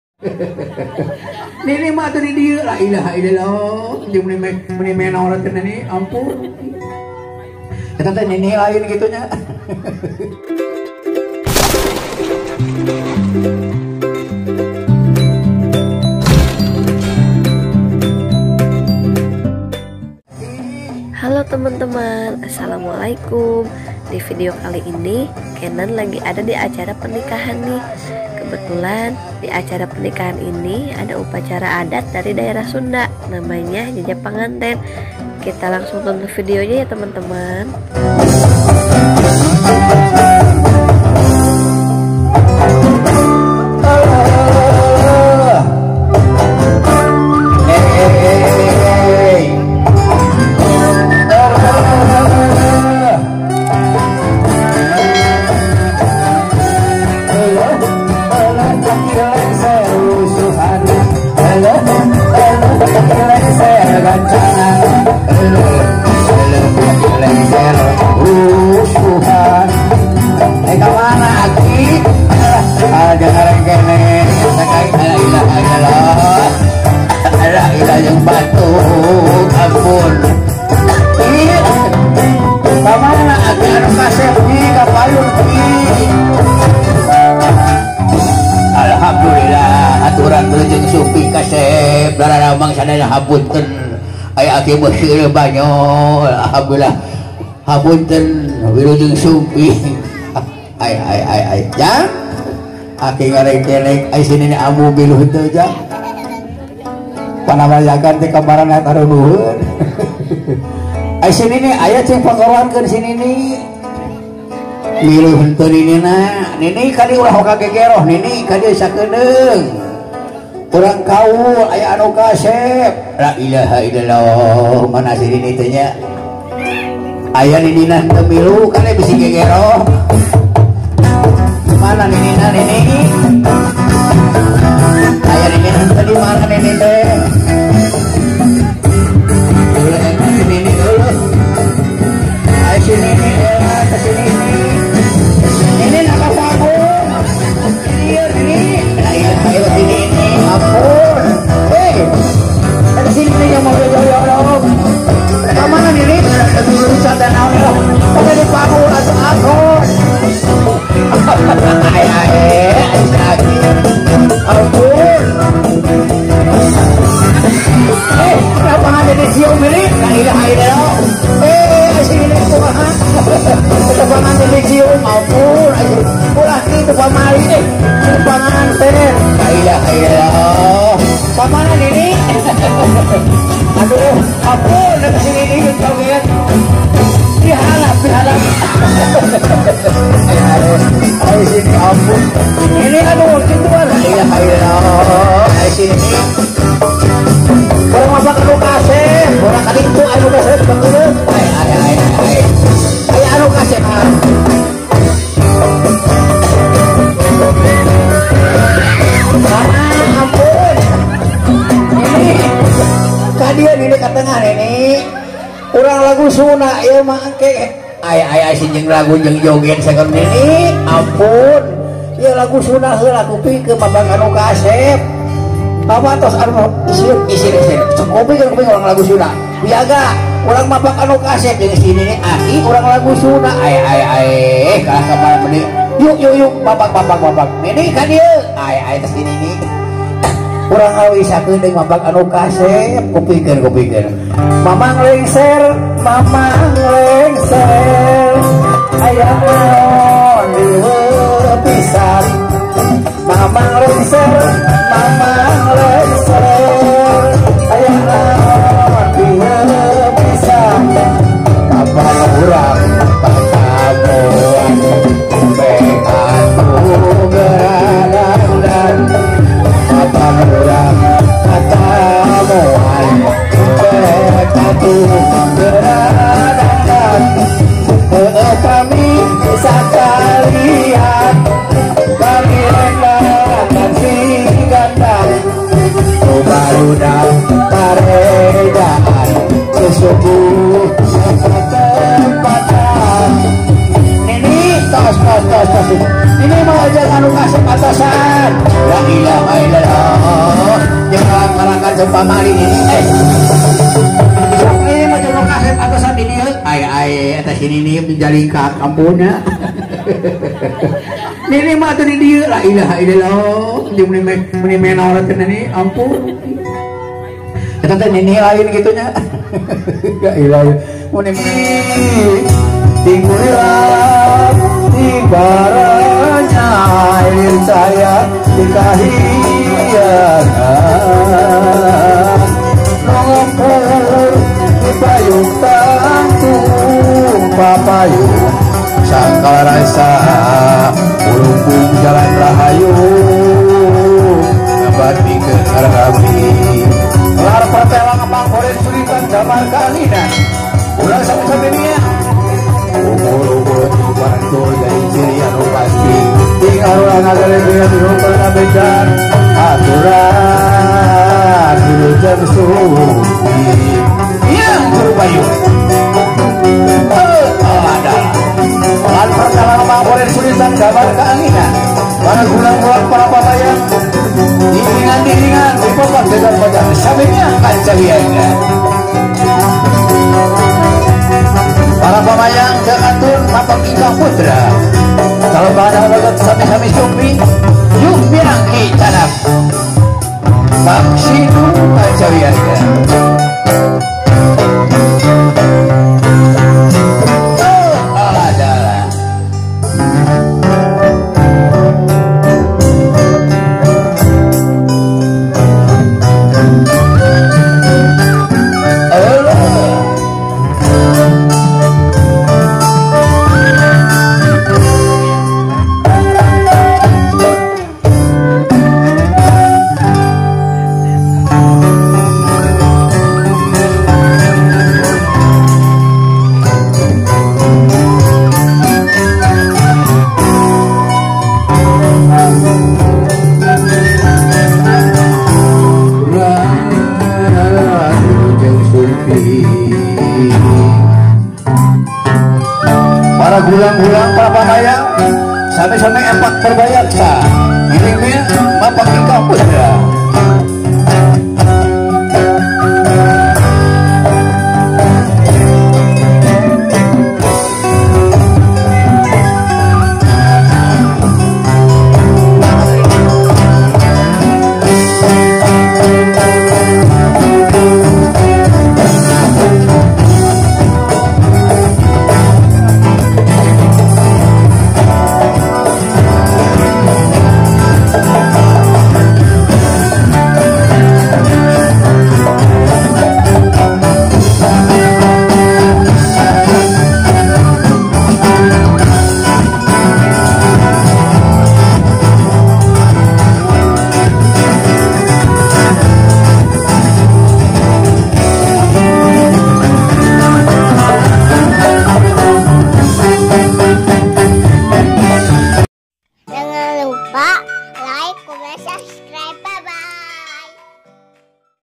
UPACARA ADAT SUNDA MAPAG PANGANTEN - Tingkah lucu aki lengser dan si nini😄
Hai teman-teman..Assalamualaikum.. di video kali ini Keenand lagi ada di acara pernikahan nih..kebetulan lagi ada upacara adat dari daerah sunda.namanya mapag panganten. dan keenand senang banget melihat tingkah lucu aki lengser dan ambu...mau tau kan? yuk langsung tonton videonya yaa.!?